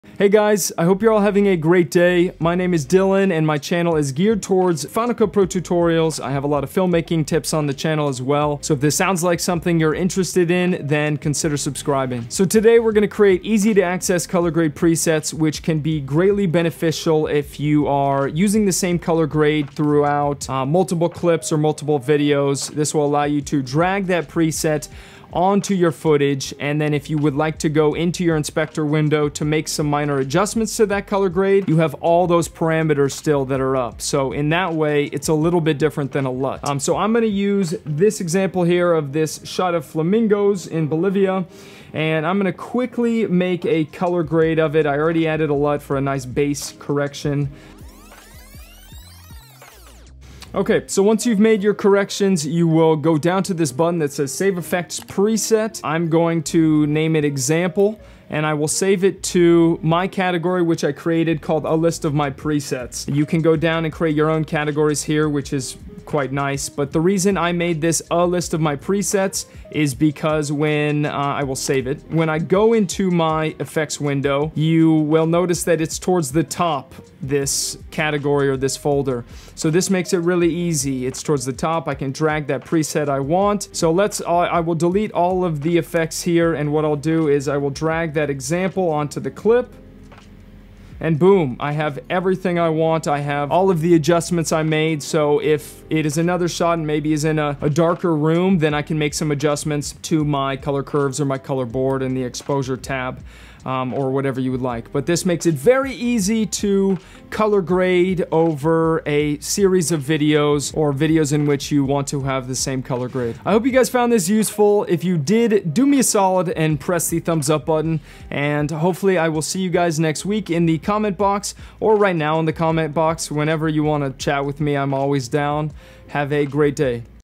Create easy to access COLOR GRADE PRESETS in Final Cut Pro
In today's Final Cut Pro X tutorial, we're going to learn how to create easy to access color grade presets in Final Cut Pro so that you can just open your effects window, drag and drop the effect, and have easy access to all of the saved color grade parameters (color board/color wheels/color curves/etc). It's important to note that saving the effects in your inspector window also saves other effects put on your clip, not just the color correction, so keep that in mind when creating the effect preset. You can also save presets for audio effects as well! I have one saved for my most used audio effects for interviews. Let me know if you have any questions!